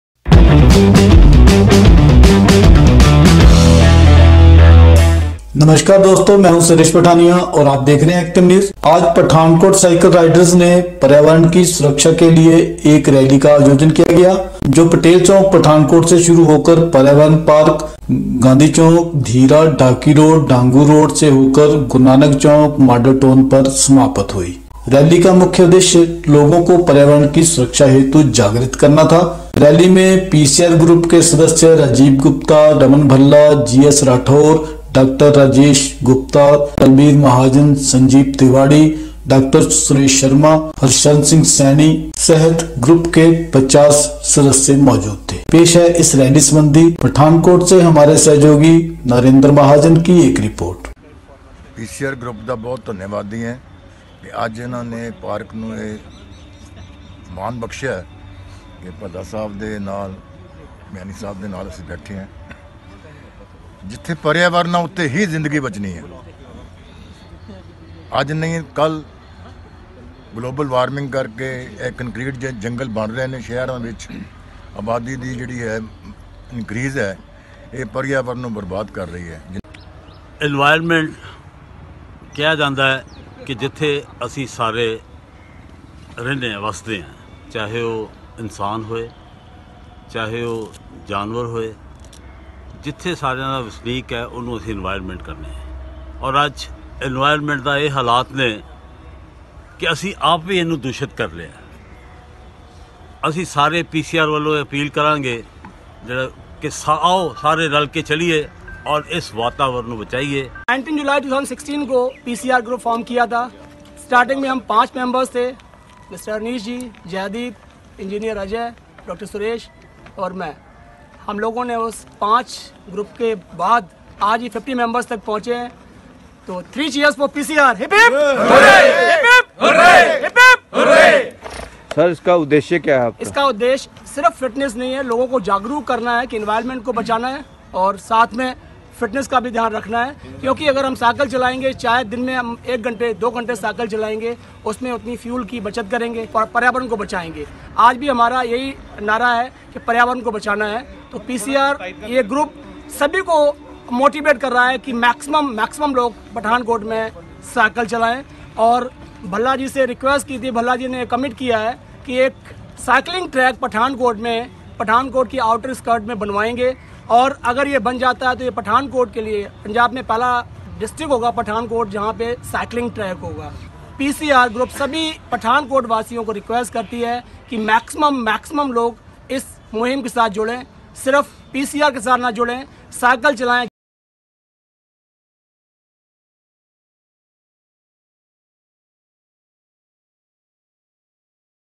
पठानकोट साइकल राइडर्स ने पर्यावरण सुरक्षा रैली निकाली
पठानकोट साइकल राइडर्स ने पर्यावरण की  सुरक्षा के लिए लोगों को जागृत करने के लिए एक साइकल रैली निकाली जिसमे पी.सी .आर.ग्रुप के पच्चास सदस्यों ने भाग लिया यह रेल्ली पटेल चौक से शुरू होकर पर्यावरण पार्क,गाँधी चौक,धीरा,ढाकी रोड ,ढांगूरोड,से होकर गुरु नानक चौक मॉडल टाउन में समाप्त हुई इस रेल्ली में राजीव गुप्ता रमन, भल्ला,जी .एस.राठौर ,डॉक्टर राकेश गुप्ता,बलबीर महाजन,संजीव तिवाड़ी,डॉक्टर सुरेश शर्मा ,हरशरण सिंह सैनी आदि मौजूद थे